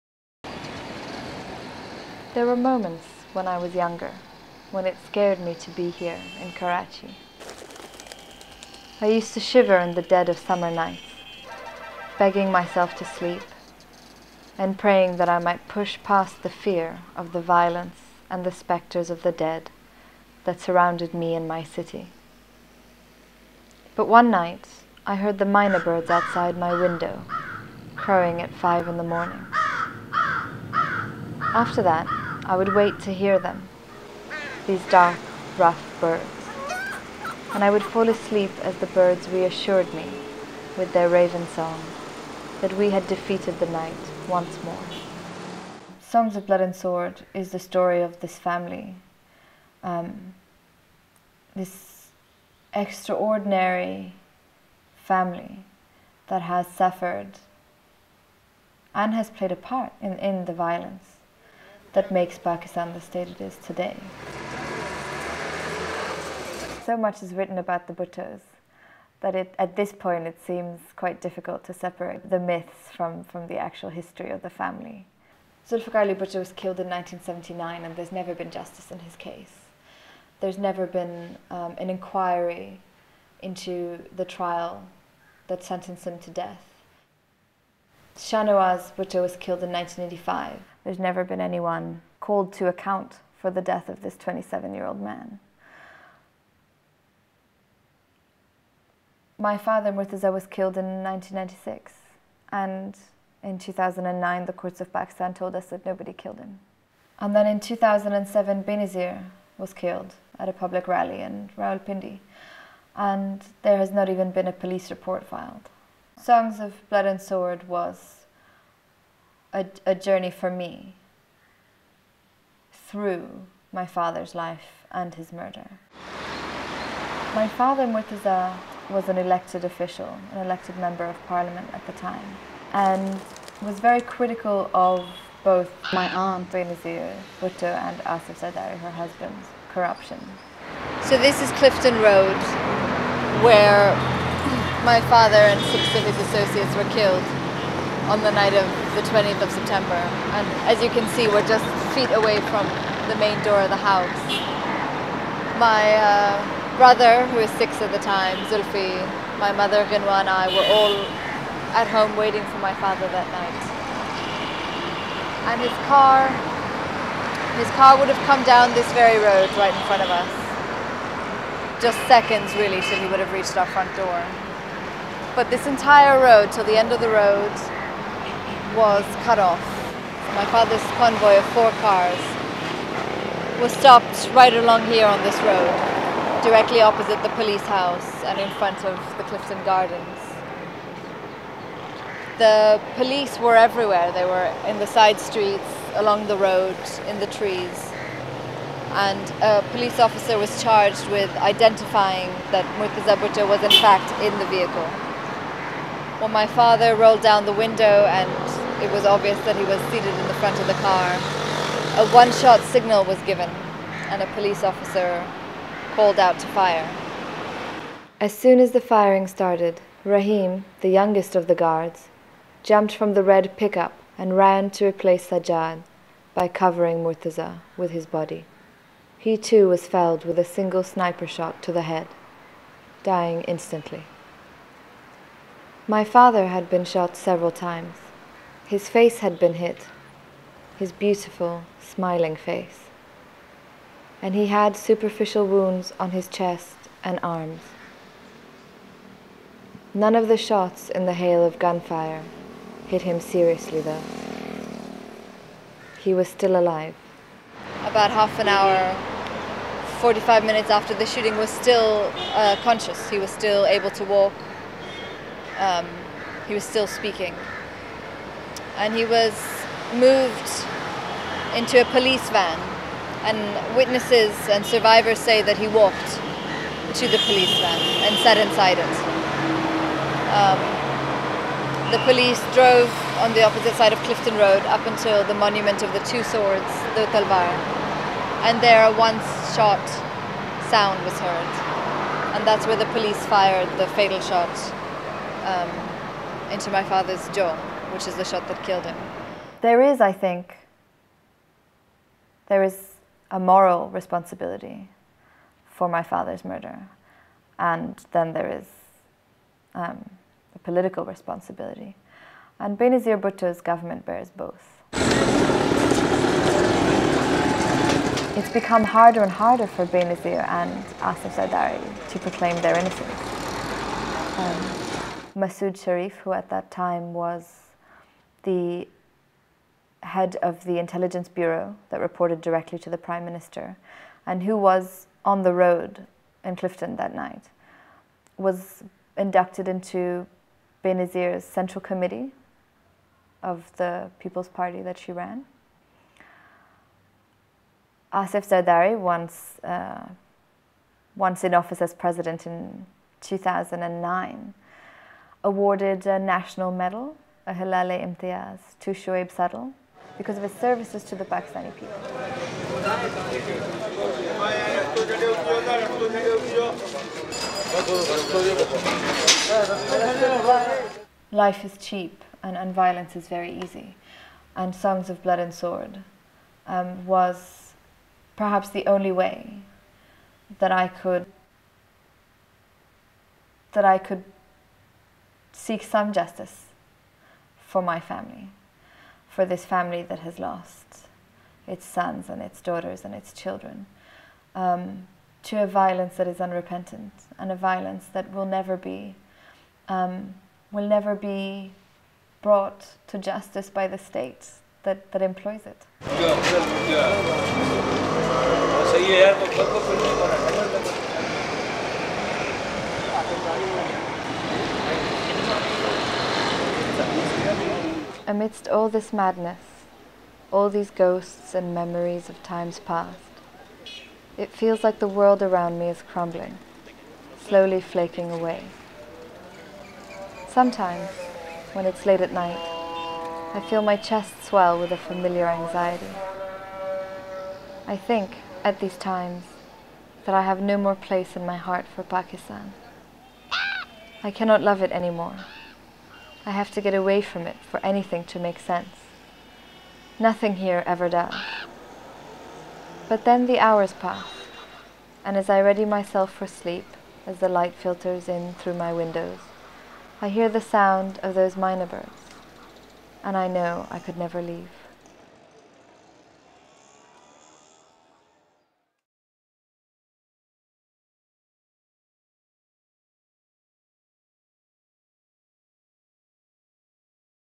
Songs of Blood & Sword - Fatima Bhutto
Songs of Blood & Sword is an important and revealing new book on the Bhutto family, a dynasty that is central to the story of modern Pakistan. Written by Fatima Bhutto, daughter of the murdered Murtaza Bhutto and niece of the murdered Benazir Bhutto - this is a book of love, family, and the desire to understand a nation riven by murder, corruption, conspiracy and division.

Published by Jonathan Cape, April 2010.